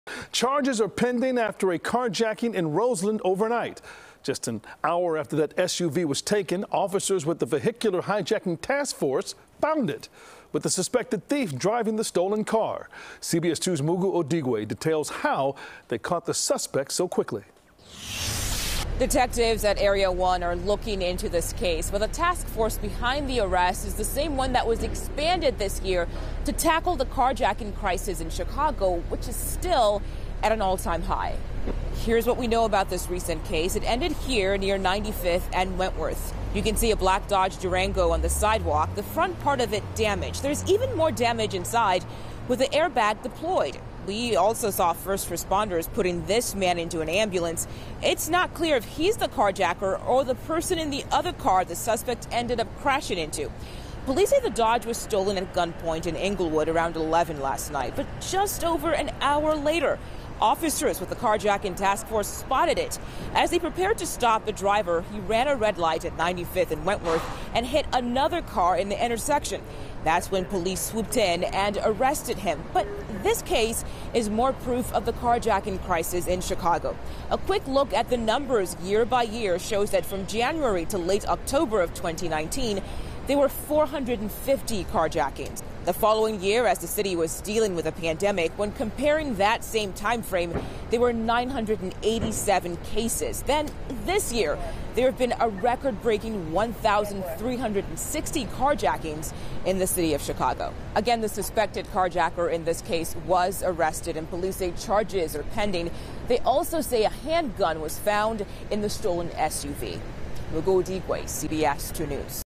Police Task Force Arrests Carjacking Suspect Hour After Vehicle Was Stolen In Englewood
The taskforce behind the arrest is the same one that was expanded this year to tackle the carjacking crisis in the city.